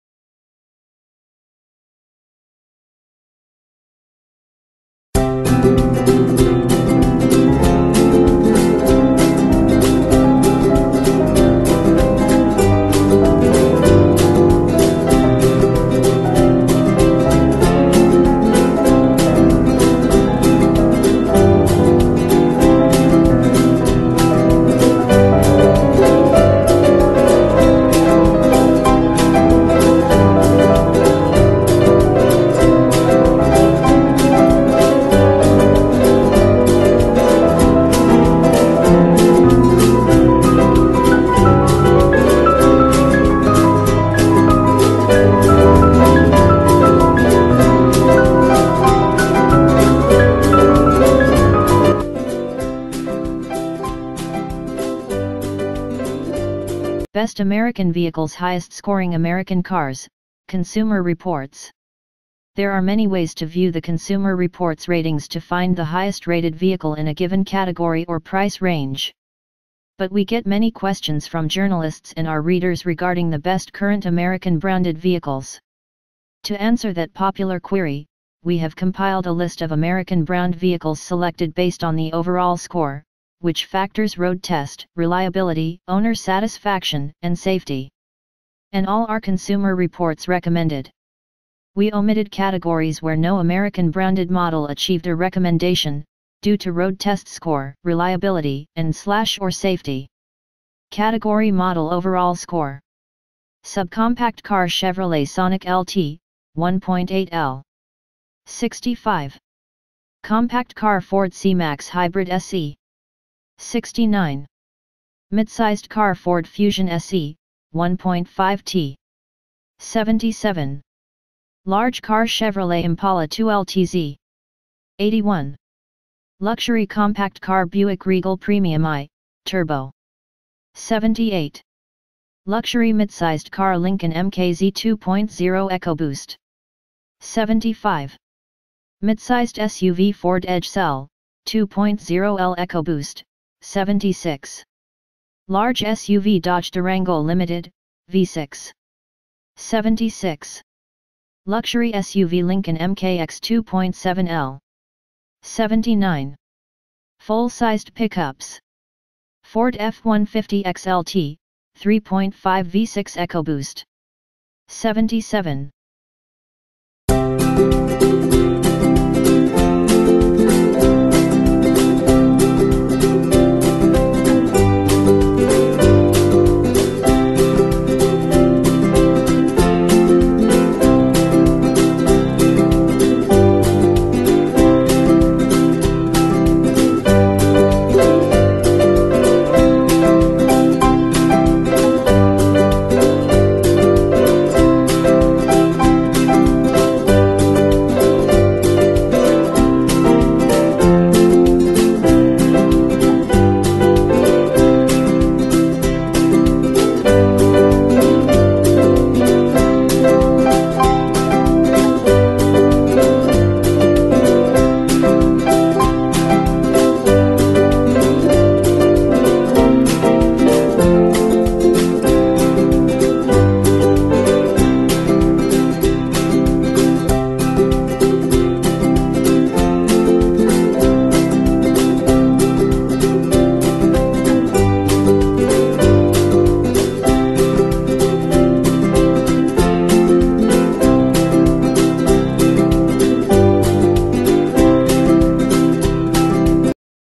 HOT MOST VIEWED Best American Vehicles Highest Scoring American Cars Consumer Reports
Best American Vehicles  Highest-Scoring American Cars - Consumer Reports
There are many ways to view the Consumer Reports Ratings to find the highest-rated vehicle in a given category or price range. But we get many questions from journalists and our readers regarding the best current American-branded vehicles.

To answer that popular query, we have compiled a list of American-brand vehicles selected based on the Overall Score, which factors road test, reliability, owner satisfaction, and safety. And all are Consumer Reports recommended. We omitted categories where no American-branded model achieved a recommendation, due to road test score, reliability, and/or safety.

Category Model Overall Score
Subcompact car Chevrolet Sonic LT (1.8L)
65
Compact car Ford C-Max Hybrid SE
69
Midsized car Ford Fusion SE (1.5T)
77
Large car Chevrolet Impala 2LTZ
81
Luxury compact car Buick Regal Premium I (turbo)
78
Luxury midsized car Lincoln MKZ 2.0 EcoBoost
75
Midsized SUV Ford Edge SEL (2.0L EcoBoost) 76
Large SUV Dodge Durango Limited (V6)
76
Luxury SUV Lincoln MKX 2.7L
79
Full-sized pickups
Ford F-150 XLT (3.5 V6 EcoBoost)
77